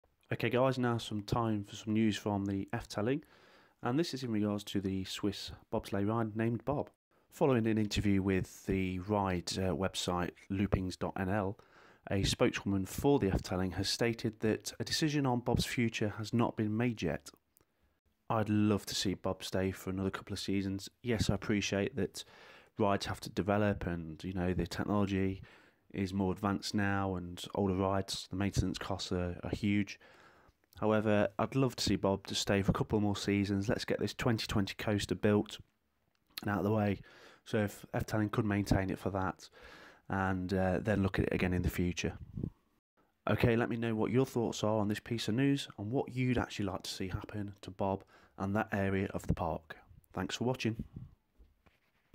Efteling Bob, Bobsleigh News - Refurb, demolish or Keep? My Thoughts
In an interview with Loopings.nl a spokeswomen has stated that Bob is coming to the end of its life and a decision on it will be made at some point in the future.  Here are my brief thoughts.